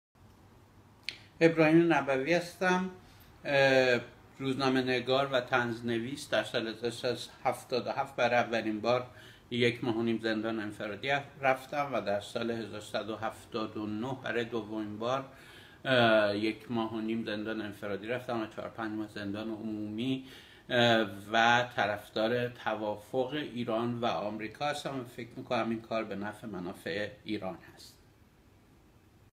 Ebrahim Nabavi, Satirist
Ebrahim Nabavi, a former political prisoner and a leading Iranian political satirist, supporting the Iran Deal.